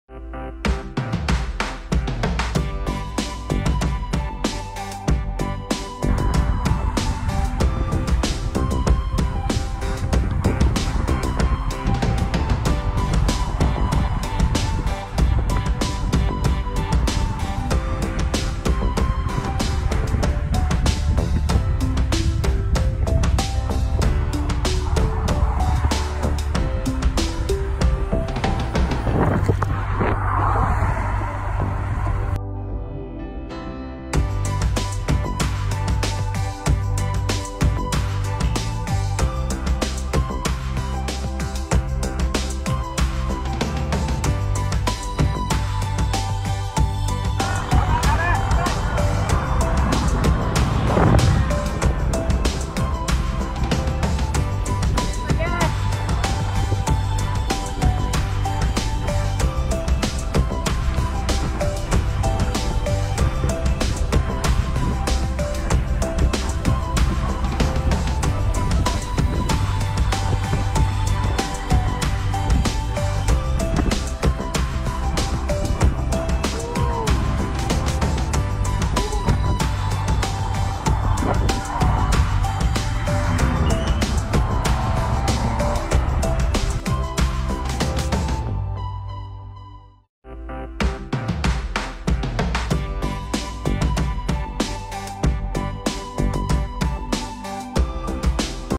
Sunday ride with P2K ... happy rolling...
Bawanut bring your balls ride...
PJ Uptown - Tanjung Malim and back ..